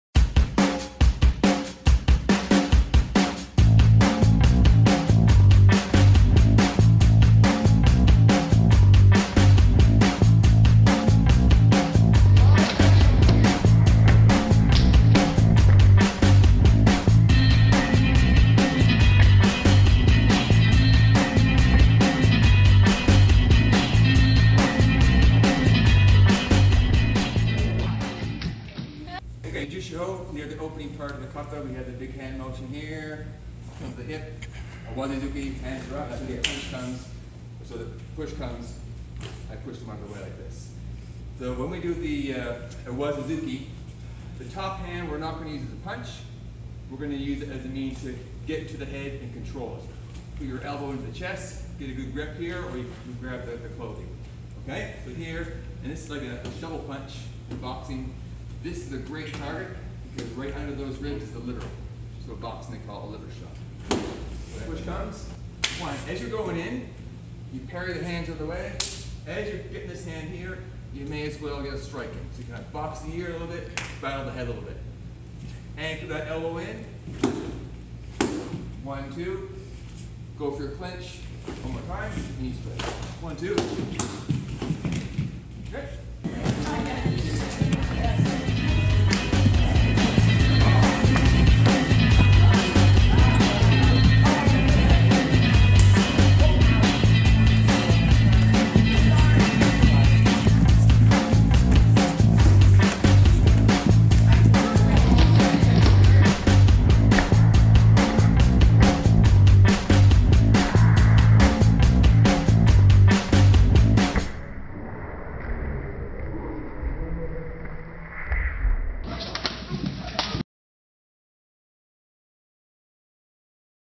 Nijushiho Shovel Hook
appliedshotokan.com This video shows how movements 4 to 6 of Nijushiho can be used to parry a two-hand shove, control the head while throwing a shovel hook, and follow with a clinch and knee strike. Note that in this part of the kata, the left leg is back while the left arm sets up for a liver shot.